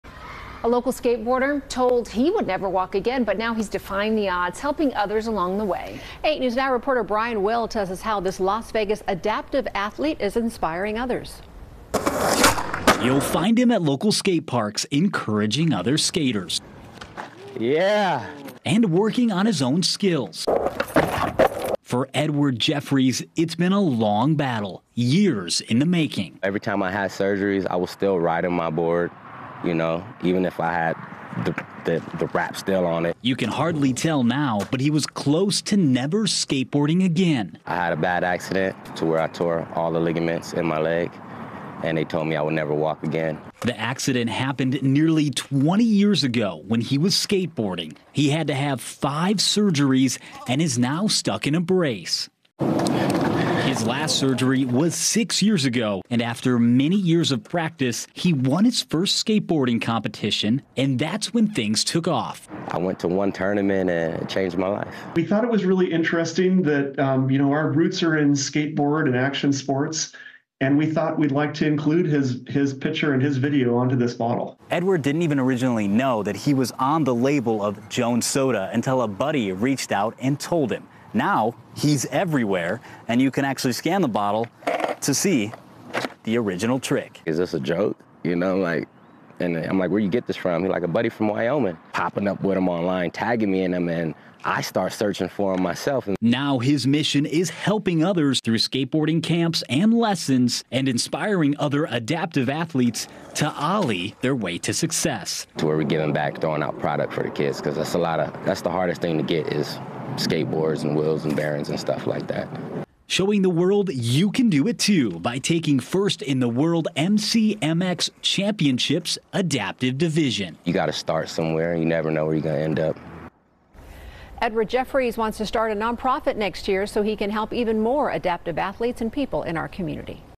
Las Vegas skateboarder 'ollies' onto soda label, inspires adaptive athletes
A local skateboarder was told he would never walk again, now he is defying the odds and helping others along the way. You’ll find him at the local skate parks encouraging other skaters and working on his skills. For Edward Jeffries, it’s been a long battle, years in the making.